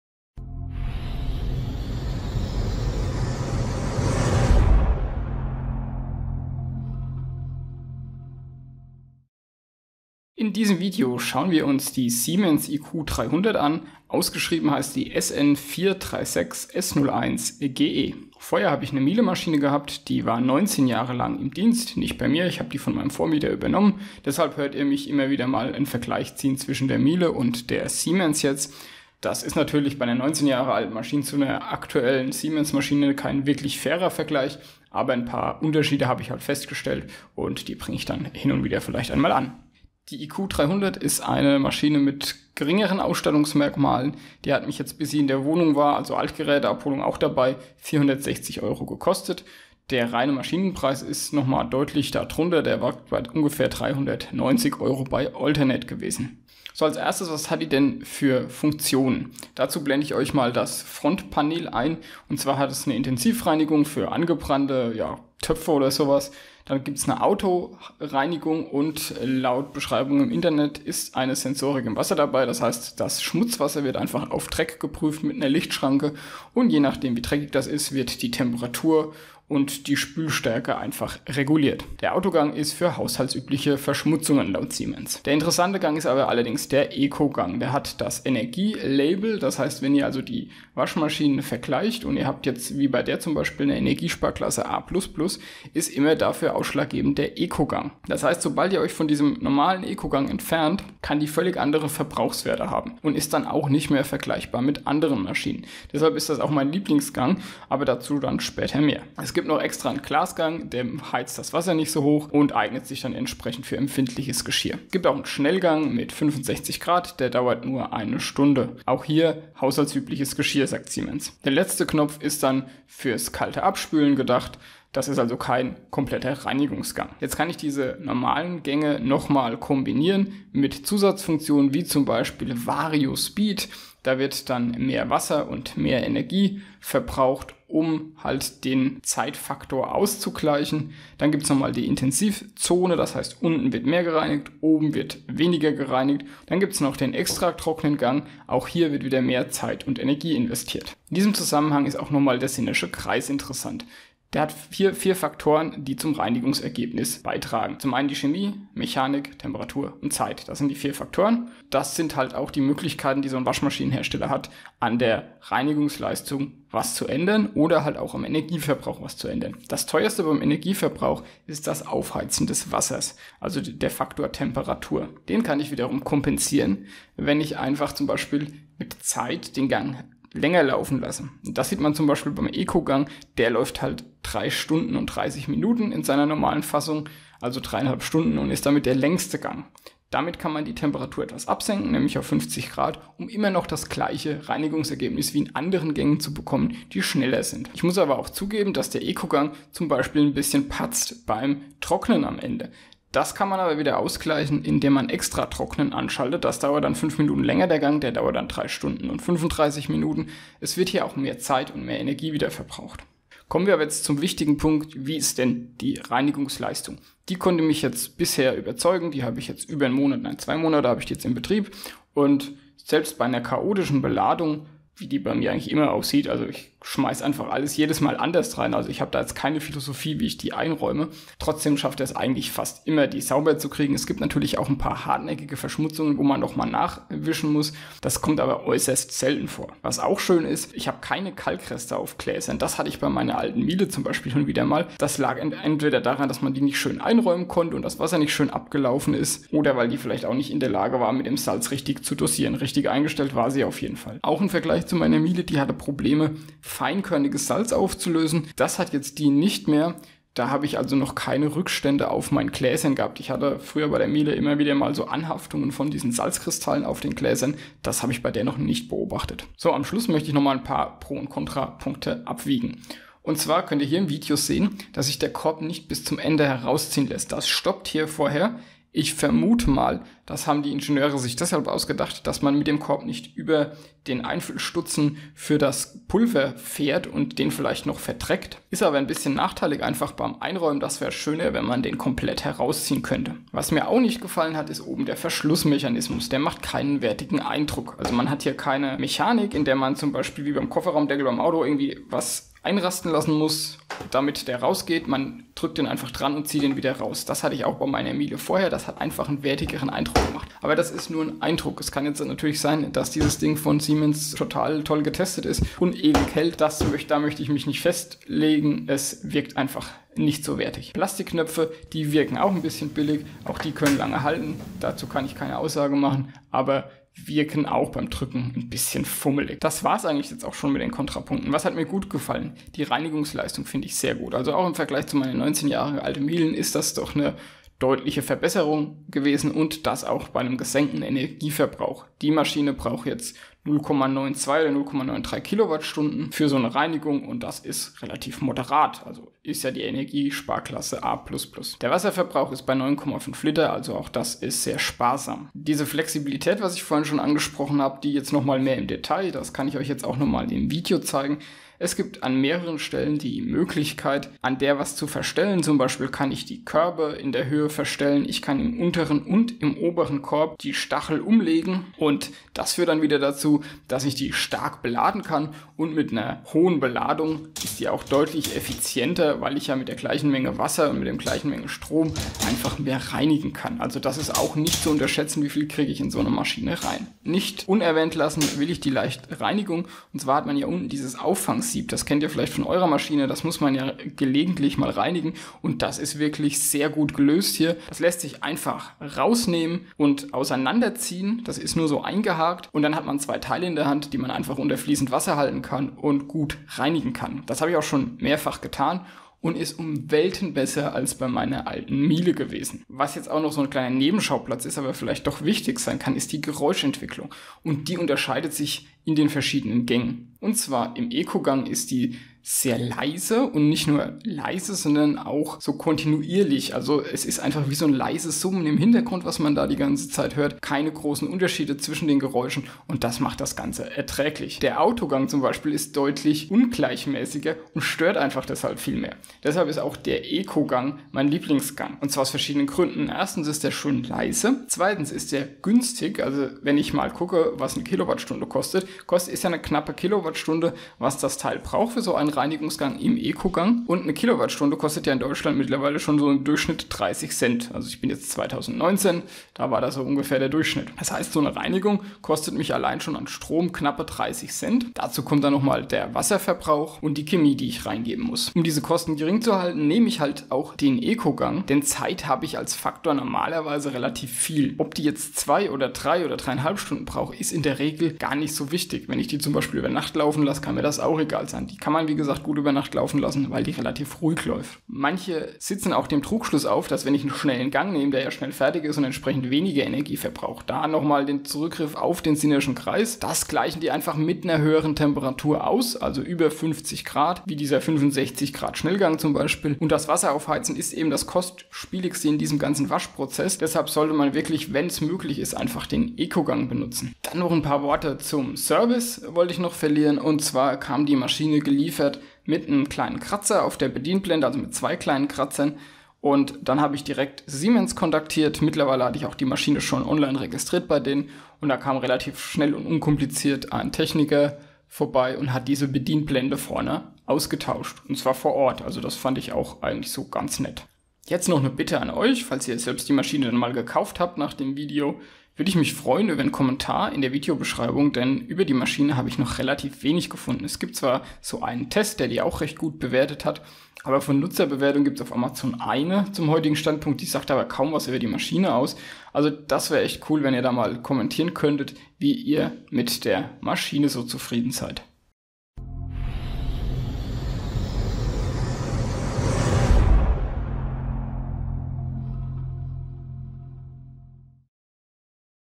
Test: Geschirrspüler Siemens SN436S01GE iQ300 (Erfahrungsbericht)
Amazon Preis (Werbung)
In diesem Video schildere ich meine Eindrücke mit der Siemens iq300 nach zweimonatiger Nutzung.

Quellen:
Chemicals-HP.jpg: Hey Paul Wasserhahn.jpg: Matthew Bowden Galileo_Thermometer_closeup.jpg: Fenners IWC_GST_ref._3707_-_dial.jpg: Jacob Seligmann derivative work: DavidG „Sinnerscher Kreis“,